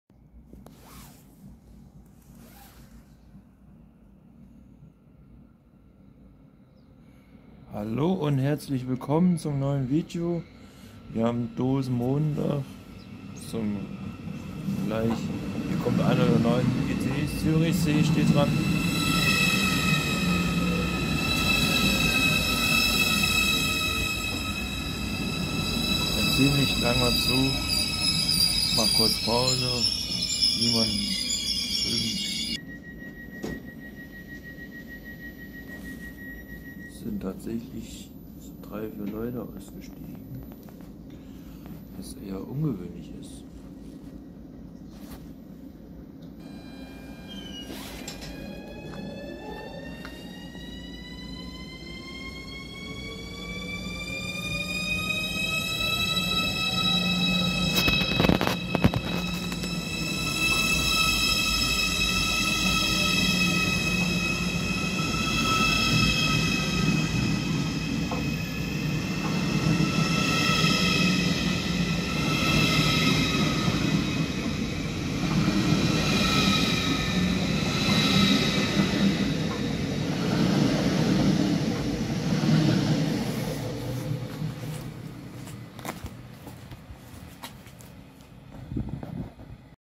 🚆 🚆 ICE 990 ROLLT AM DOSENMONTAG 2023 NACH HAMBURG 🍻 🍻
Tja, nachdem der ICE um kurz nach 18 Uhr in München startete, fuhr er verspätet im Hersfelder Bahnhof ein. ) Lt. der Bahn-App in der Nacht hatte der Zug 16 Minuten Verspätung; als Grund wurde WARTEN AUF ANDERE FAHRGÄSTE genannt.

melden.